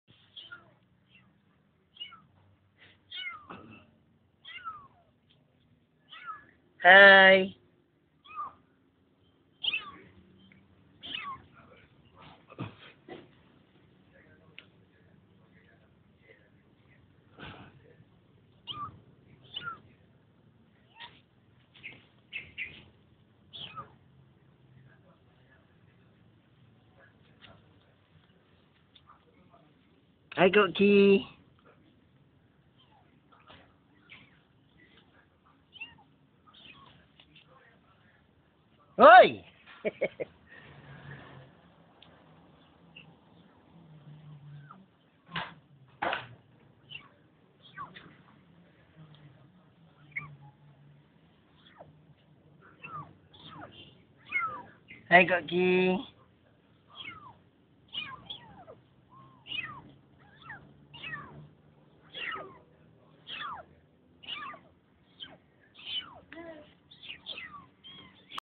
Kokki and babies
Since i already moving to my new house,and have garden .every days i will lets few cats play on the grass. shakikkoki bring her babies play on the grass. they look happy.